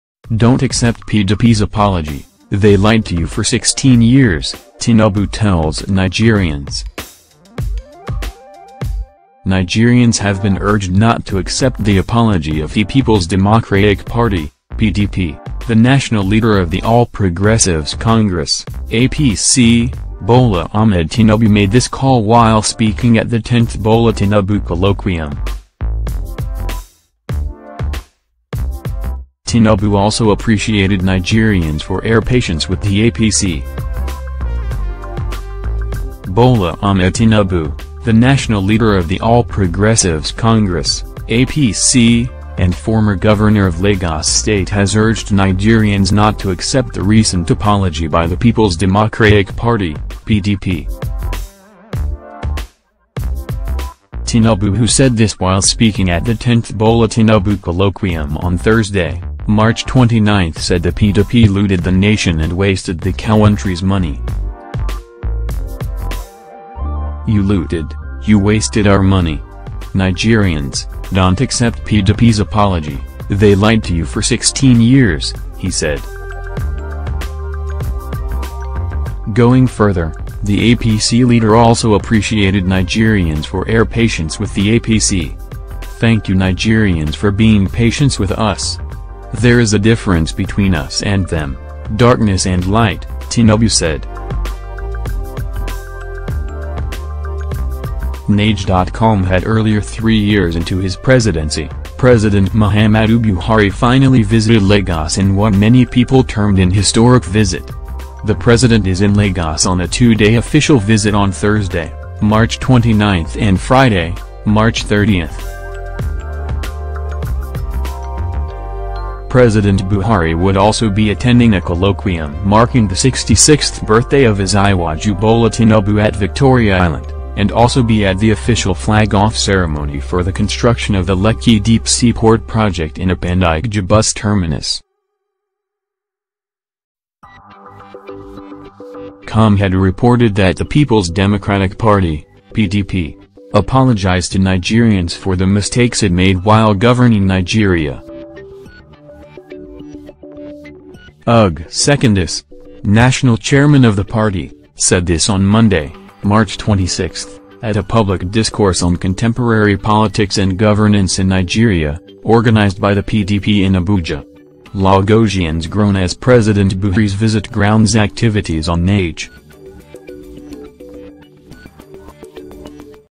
Don't accept PDP's apology, they lied to you for 16 years - Tinubu tells Nigerians
- Nigerians have been urged not to accept the apology of he Peoples Democraic Party (PDP)